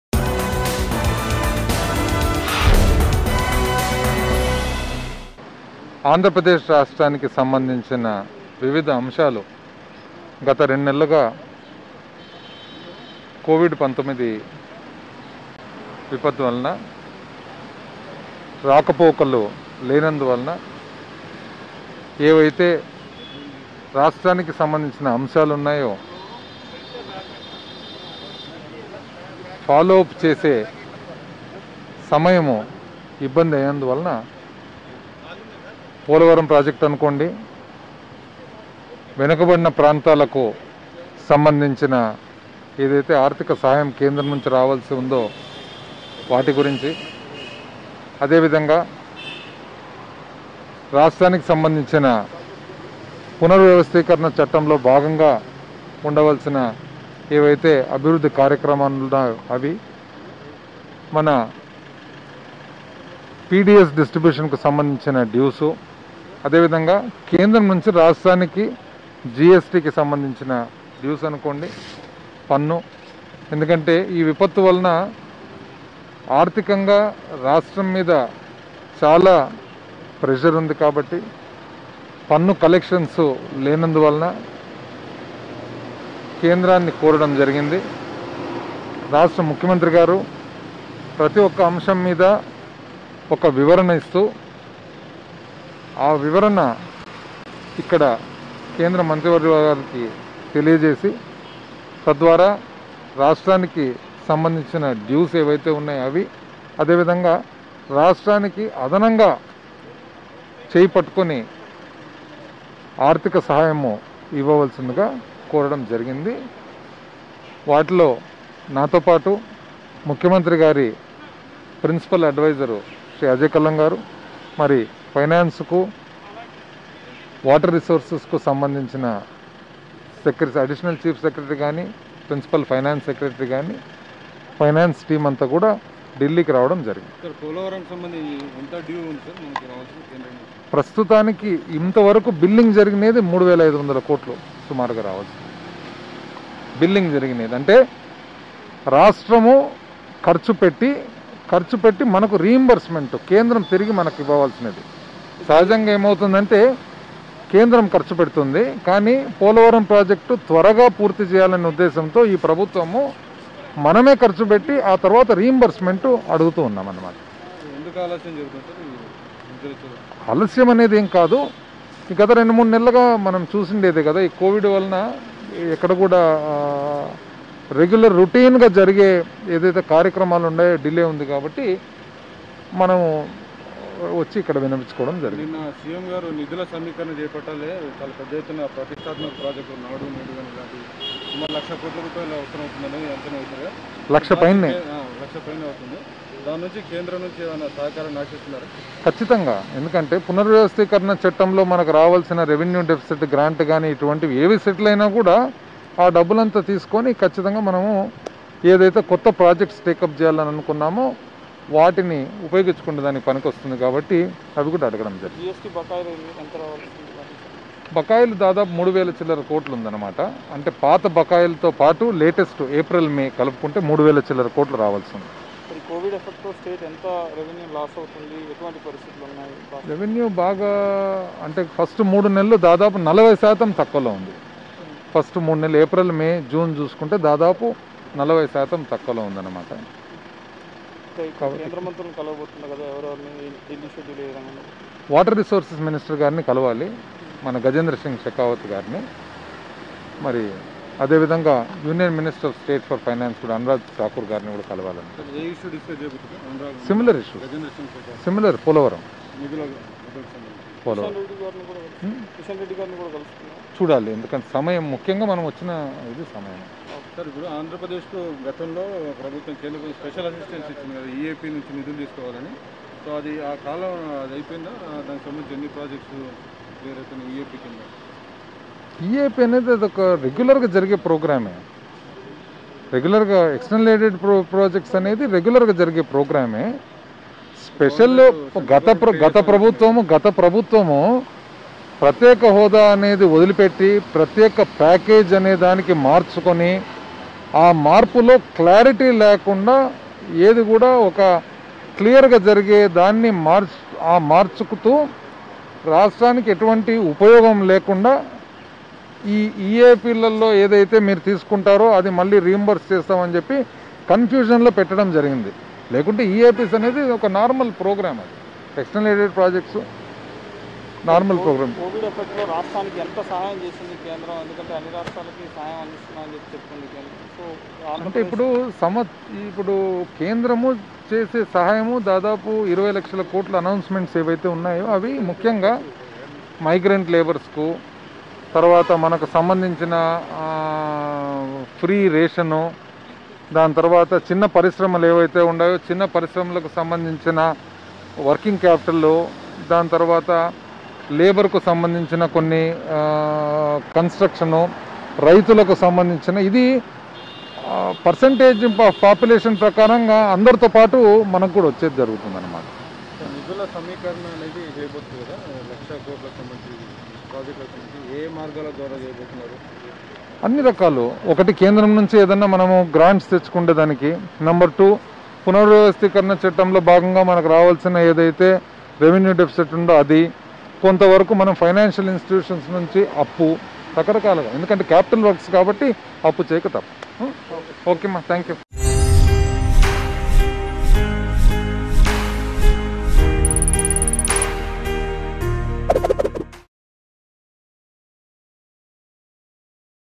Minister Buggana Rajendranath Gives Mind Blowing Answer To Reporter In Delhi | AP News | Mango News
Minister Buggana Rajendranath Gives Mind Blowing Answer To Reporter In Delhi.